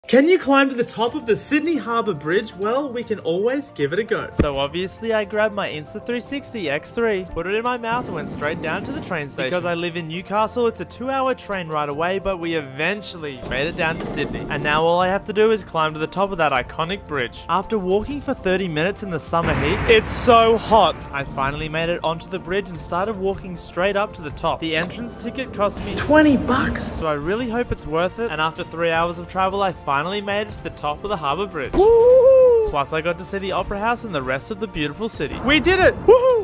CLIMBING UP Sydney Harbour Bridge in Australia!
Exploring Sydney with @insta360 !!!